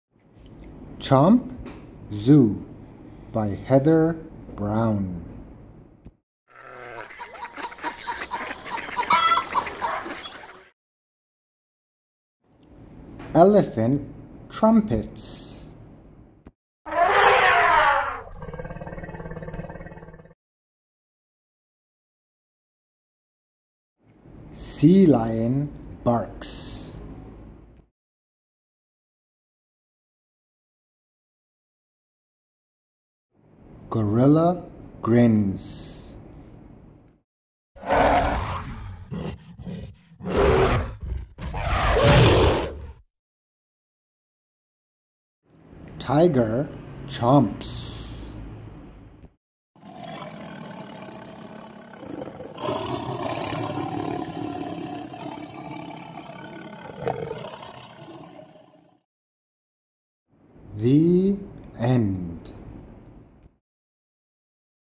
Chomp! Zoo - Heather Brown
A cute board book for babies about animals and their teeth! The board book really chomps and sounds were added to make it sound like animals are really here!